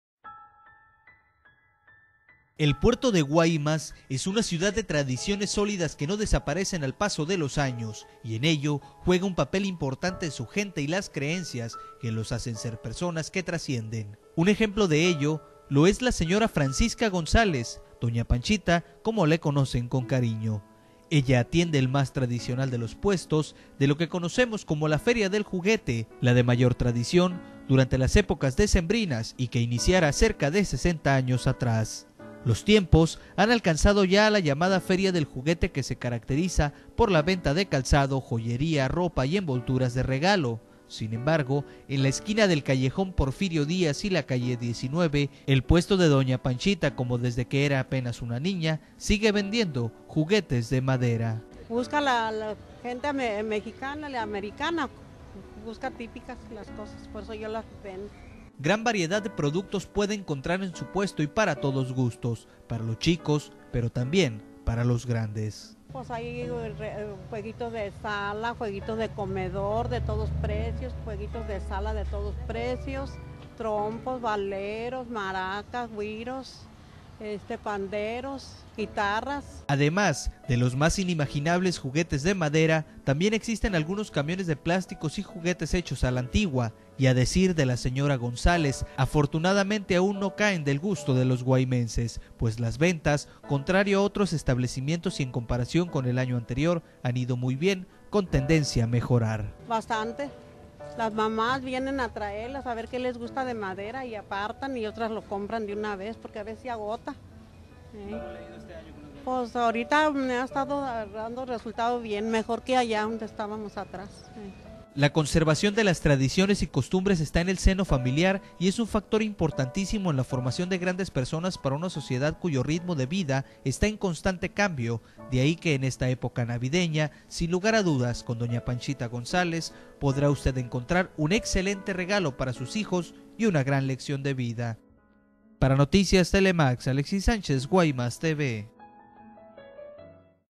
Preservan tradición con la venta de juguetes de madera en Guaymas
La señora Francisca González o doña Panchita como le conocen, atiende el único puesto de juguetes tradicionales en la "Feria del Juguete" de los locatarios del Mercado Municipal en Guaymas, Sonora como una convicción personal de conservar las tradiciones.